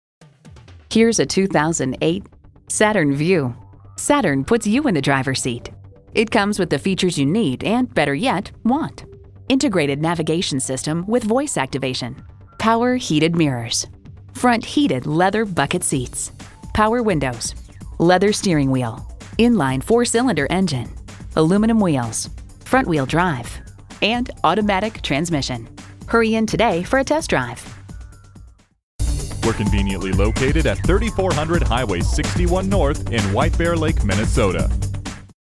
Used 2008 Saturn VUE Saint Paul White-Bear-Lake, MN
Year: 2008
Make: Saturn
Model: VUE
Trim: FWD 4dr I4 Green Line *Late Avail* 2.4l 4cyl
Engine: 146 L 4 Cyl. Cyl.
Transmission: 4-Speed A/T
Color: Techno Gray
Interior: Gray
Mileage: 115862
Stock #: W87491A
VIN: 3GSCL93Z68S728112
We sold it brand new when we were a Saturn store. One owner, accident and damage free CARFAX report. Mild Hybrid Saturn Vue. Very much above average in looks and feel. This one drives nice and seems to have lots of life left in it. Nearly everything has been done on this SUV in our shop over the years including nearly all of the oil changes.

Address:
3400 Highway 61 North